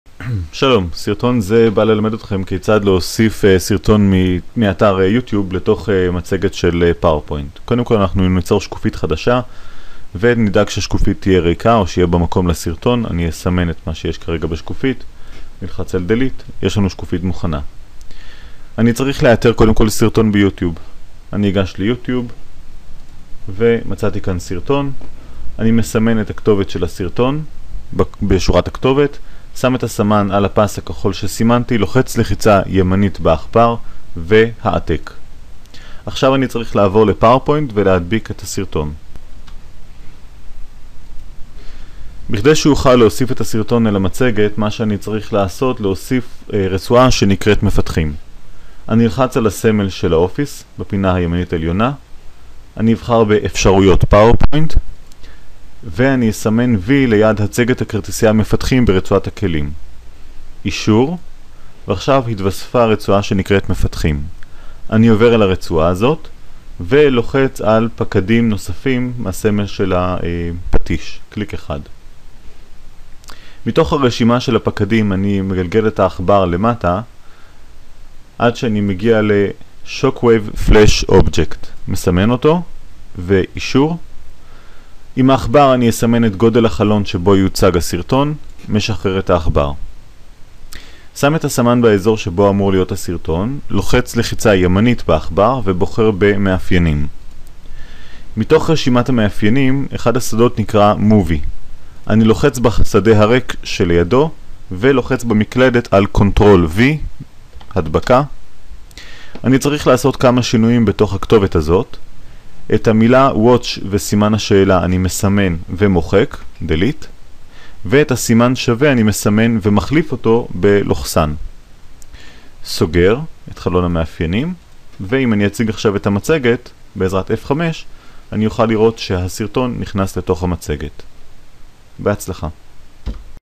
הטמעת סרטון במצגת
כיצד להוסיף סרטון מיוטיוב לתוך מצגת פאוורפוינט גירסת 2007